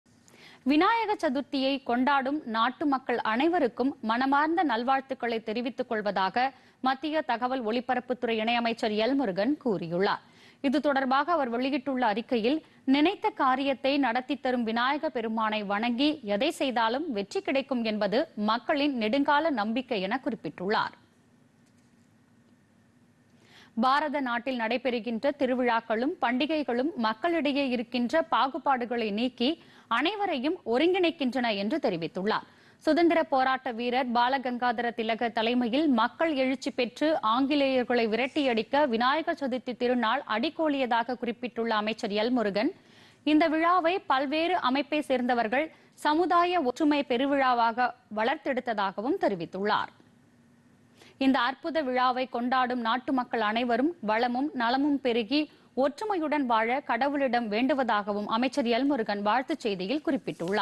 VinayakaChaturthi | விநாயக சதுர்த்தி - மத்திய இணையமைச்சர் எல் முருகன் வாழ்த்து
விநாயக சதுர்த்தியை கொண்டாடும் நாட்டு மக்கள் அனைவருக்கும் மனமார்ந்த நல்வாழ்த்துக்களை தெரிவித்துக் கொள்வதாக மத்திய தகவல் ஒலிபரப்புத்துறை இணையமைச்சர் எல் முருகன் கூறியுள்ளார்